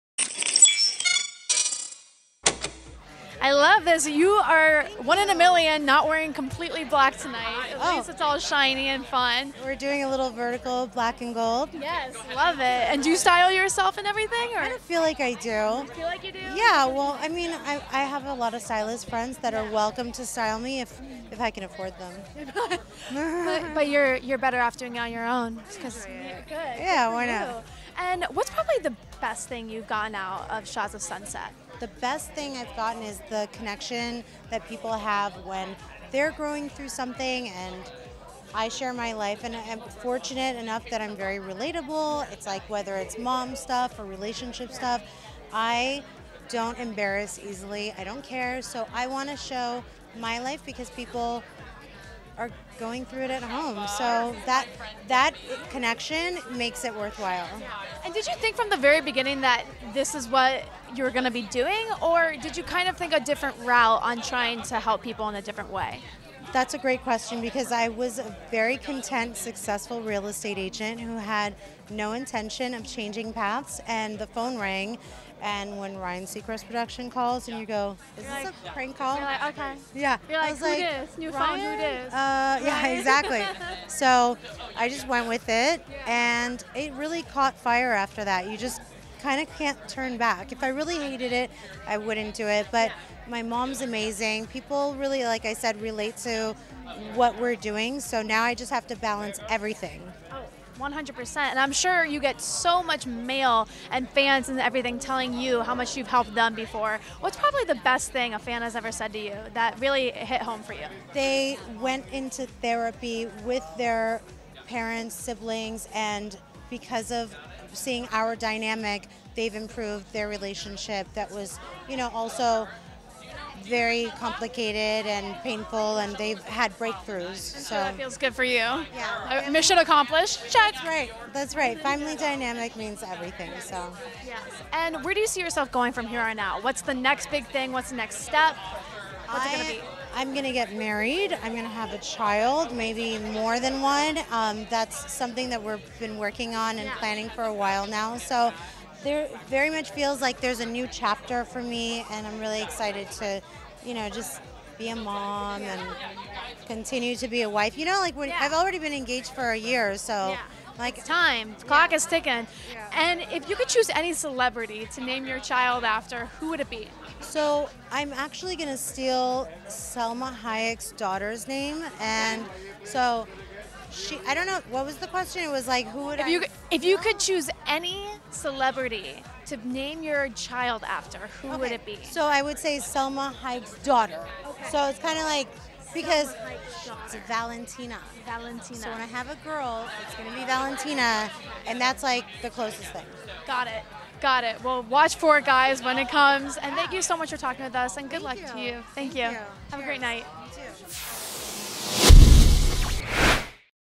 Mercedes Javid Interview 2017 LaPalme Magazine's Fall Cover Party Red Carpet
Mercedes "MJ" Javid interview on the red carpet at the LaPalme Magazine’s fall cover release party held at the Nightingale in Los Angeles, California USA on November 8, 2017 - Interview by Madison Makely